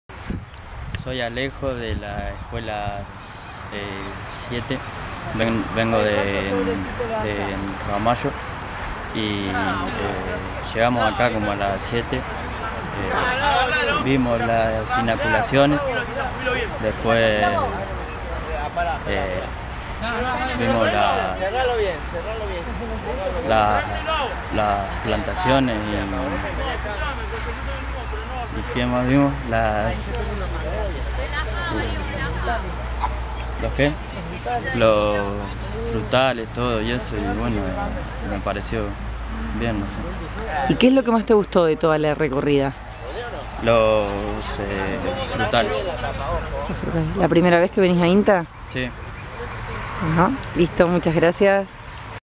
Alejo - Escuela Secundaria 7 - El Paraíso (Pdo. de Ramallo)
Estuvo en la Vidriera Tecnológica 2016 y dejó su testimonio.